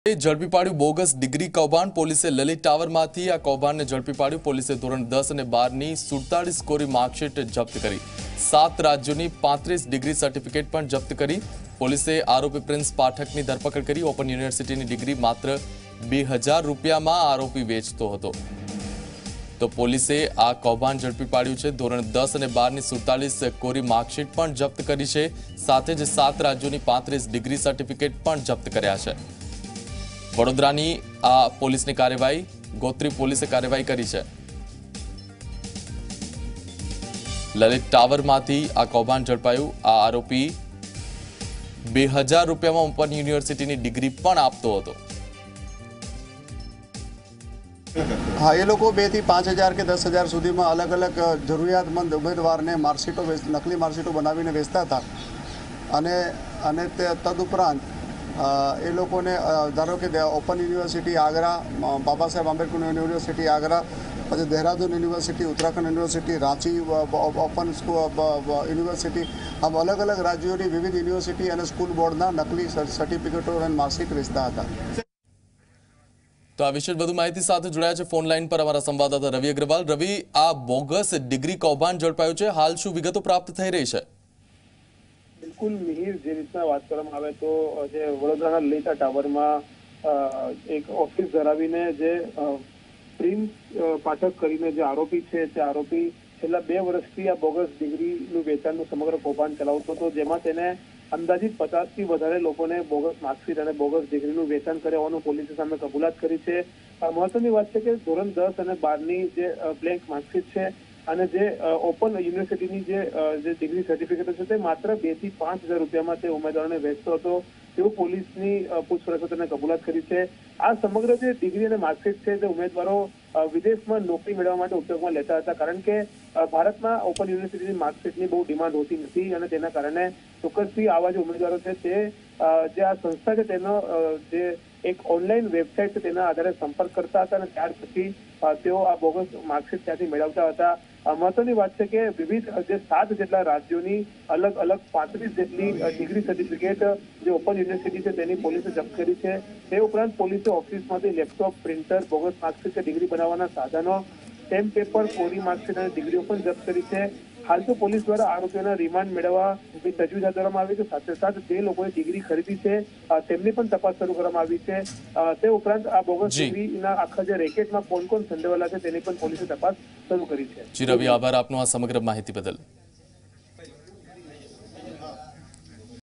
Vadodara : Gotri Police found bogus degree scam from Lalit Tower |Zee 24 Kalak
Vadodara : Gotri Police found bogus degree scam from Lalit Tower